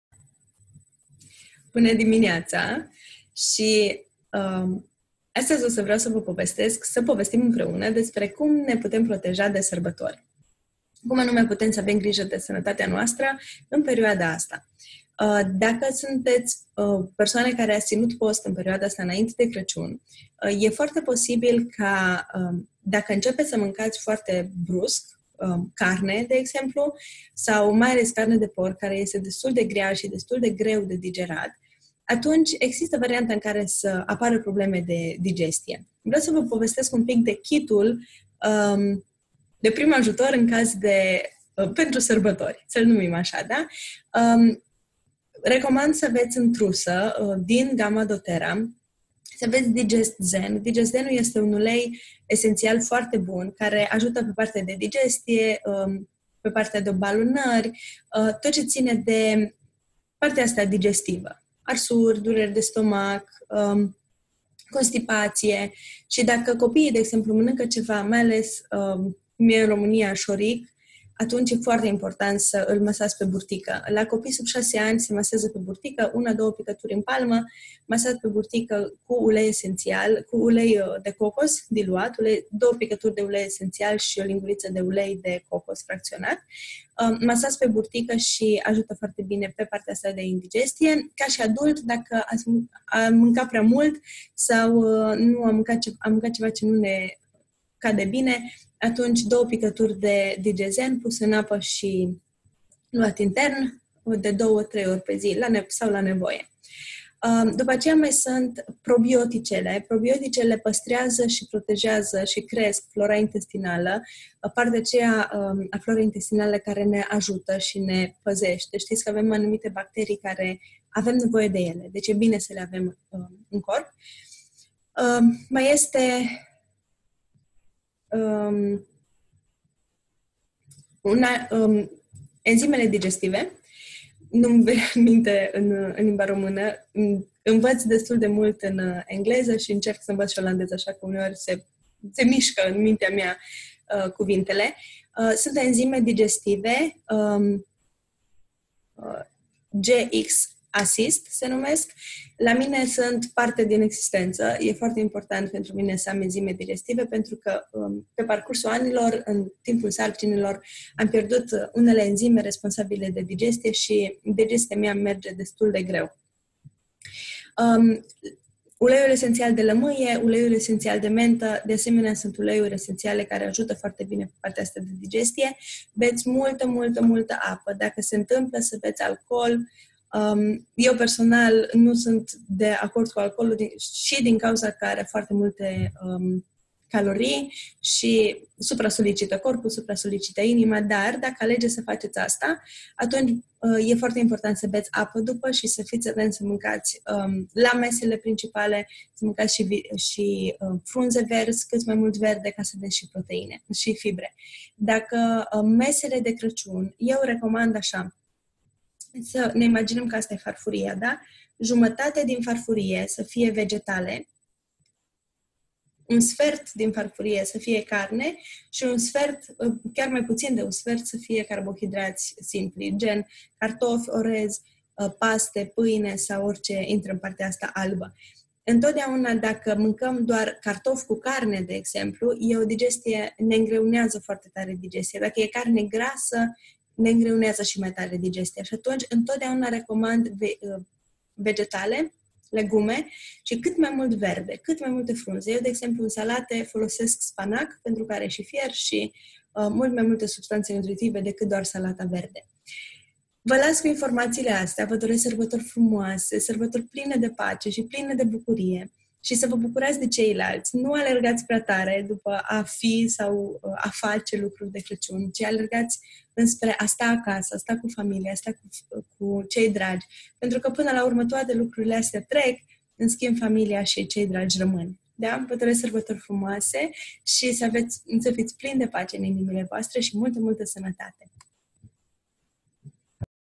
Trusa de prim jutor pentru Craciun in caz de indigestie
Pe langa grija pentru ce punem in gura, este bine sa avem si ajutoare de nadejde care sa ne salveze din situatii neplacute. Pentru asta avem doTERRA pe aproape.